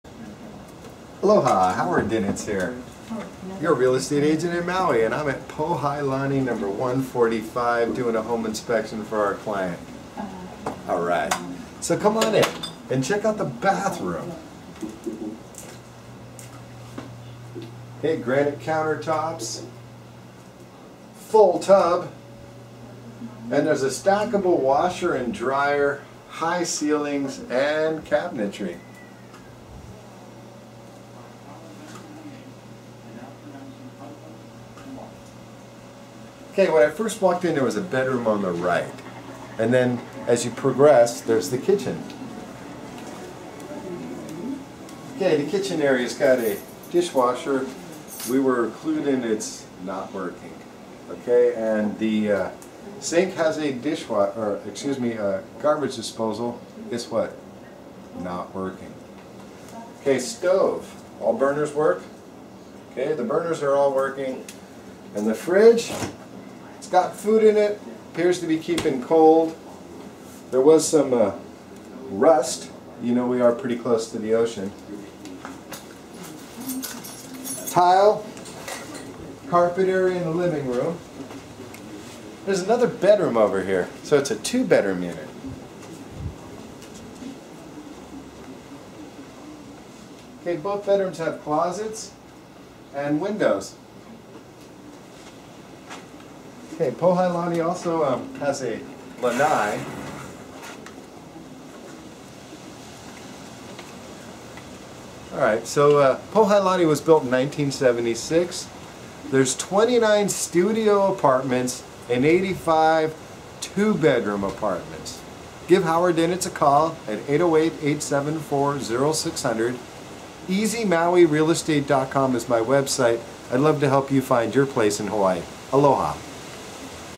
Pohailani Maui condo Lahaina
Howard Dinits your buyers agent previews condo for sale for buyer client Pohailani Maui 2 bedroom condo in Lahaina